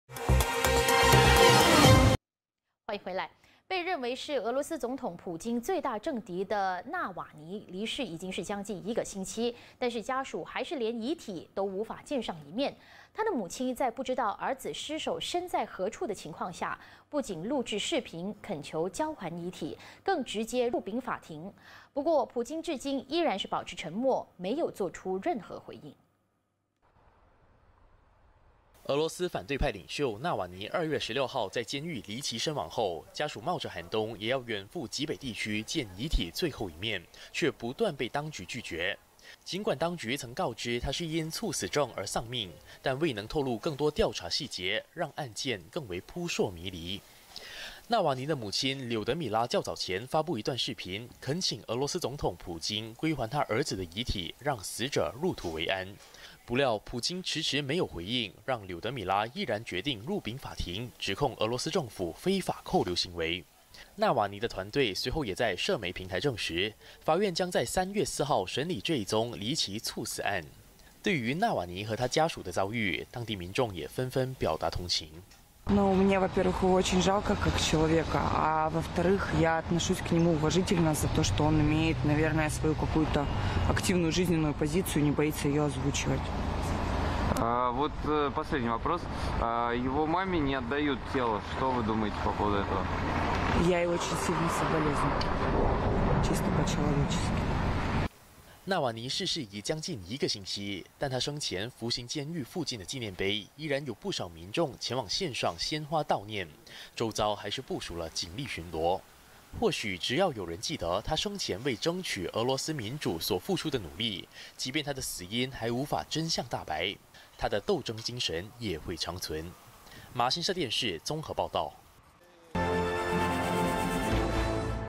20240222 纳瓦尼家属领尸之路屡碰壁 母亲喊告法院3月4日开审
俄罗斯反对派领袖纳瓦尼，离奇逝世近一星期，但家属至今连遗体都无法见上一面；他的母亲更发布视频，恳请俄罗斯总统普京归还遗体。

她也直接入禀法院指控俄罗斯政府"非法扣留行为"。